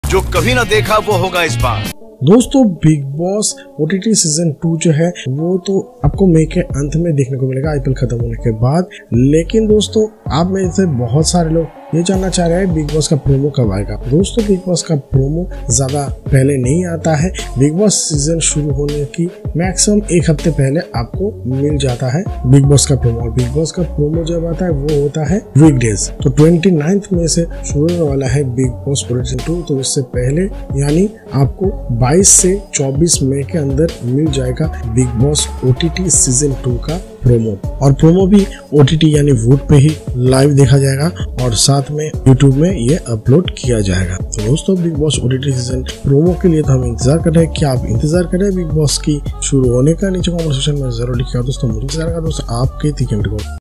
Bigg Boss OTT Season 2 Promo Release date has been confirmed kab ayega promo
इस दिन से शुरू होगा Bigg Boss OTT 2 इस दिन आएगा Bigg Boss OTT 2 का Promo

Bigg Boss 16, is the sixteenth season of the Indian reality TV series Bigg Boss. It premiered from 2022 on Colors TV.Salman Khan hosted for the thirteenth time.

Bigg Boss OTT Got Cancelled